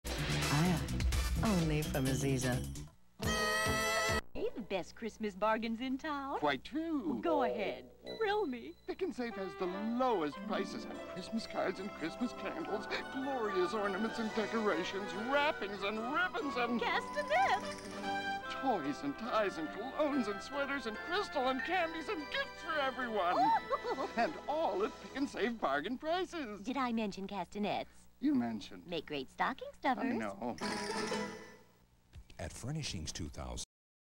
1988 Pic 'n' Save animated television commercial
"The best Christmas discounts in town" are listed in this animated 1988 television commercial for Pic 'n' Save featuring the same couple seen in our separate upload, "1989 Pic 'n' Save animated television commercial."